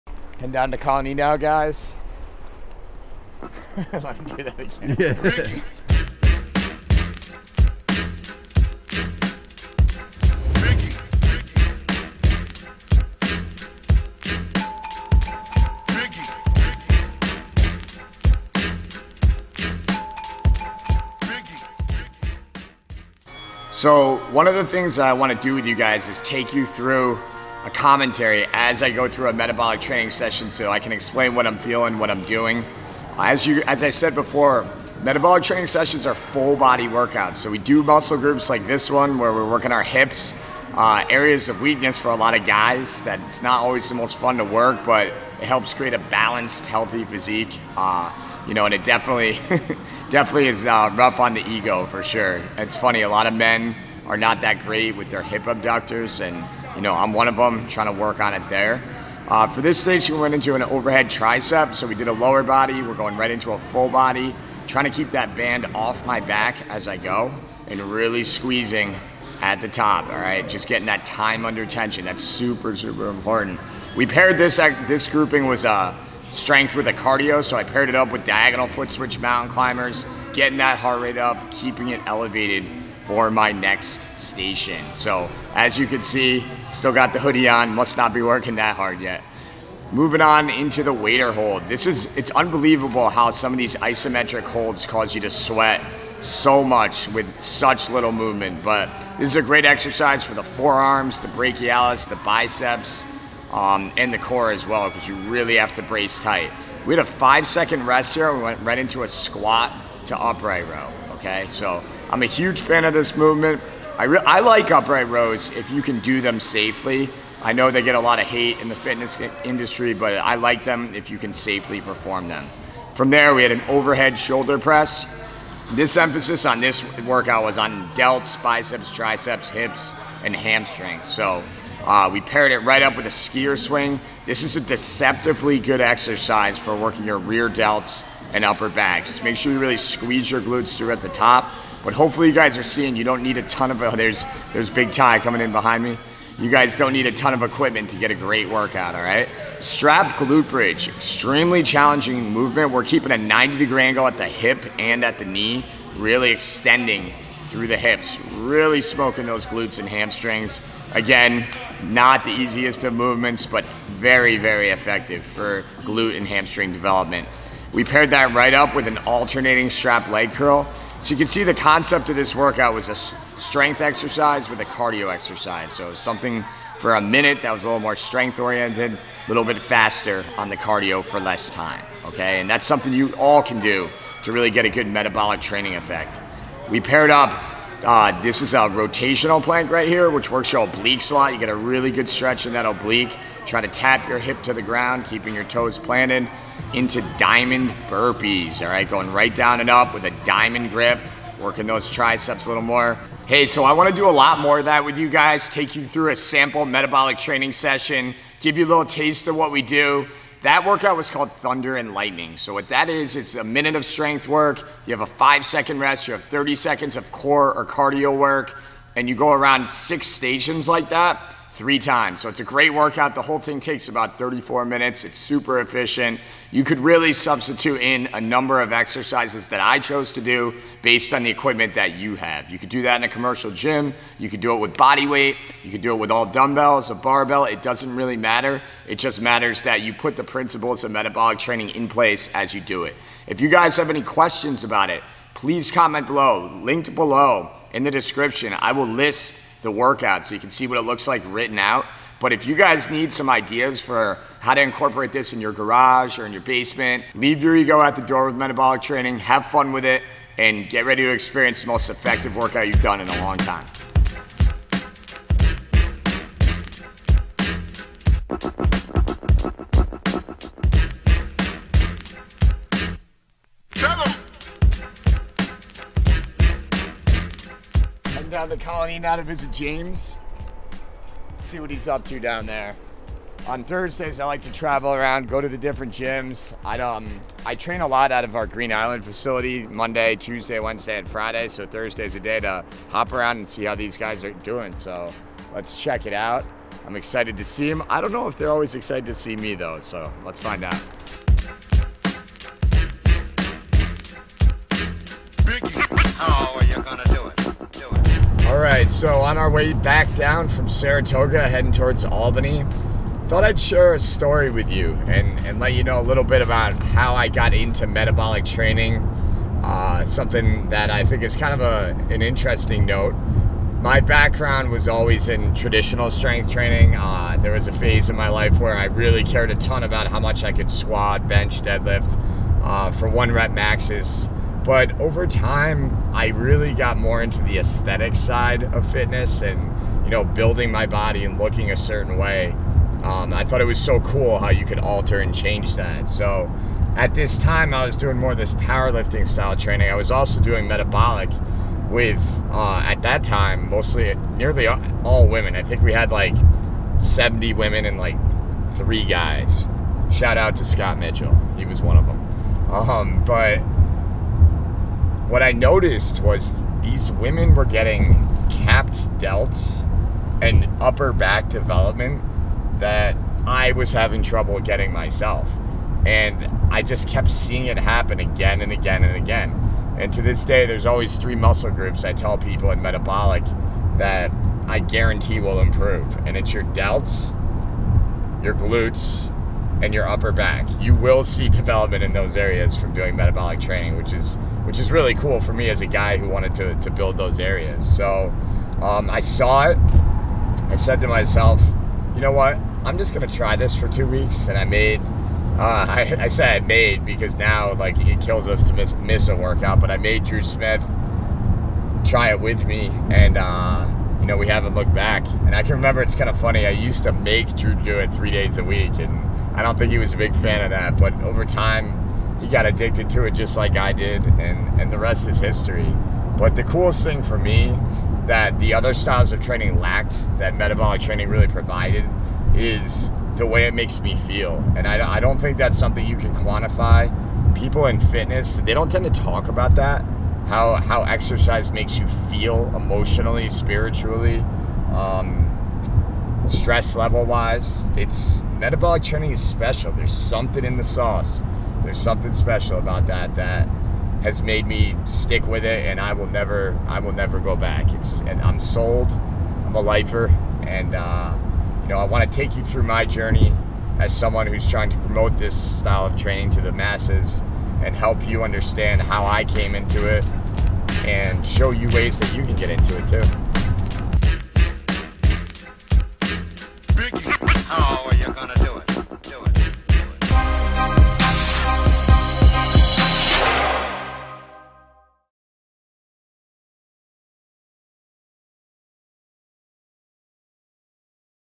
Vlog 3 - Let's Talk - Matt Phelps
In this video, I walk you through my recent workout with some commentary and talk about what different muscle groups we focus on and how I structure my workouts at my gym. Also, I share a story from the beginning and how Metabolic became what it is today...and as promised, here is the template for the workout below!
Description: Perform the first movement for 1:00, rest for :05, then perform the second movement for :30. Take :20 to transition into the next movements. Perform this whole workout 3 times.

1) Band Triceps -- Diagonal Foot Switch Mt. Climbers
2) KB Waiter Hold -- KB Squat to Upright Row
3) DB Shoulder Press -- DB Skier Swing
4) Strap Glute Bridge -- Alternating Strap Leg Curls
5) Twisting Plank -- Diamond Burpee
6) Plate Band Monster -- Band Squat Jacks